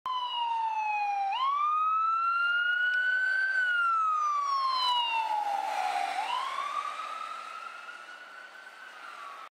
Lutheran EMS Responding
Captured in this video is a Ford Ambulance from Dukes EMS in Miami County, IN responding to a call.
The camera I used to film this video is a Nikon COOLPIX L810

Apparatus Info:
15-18? Ford Ambulance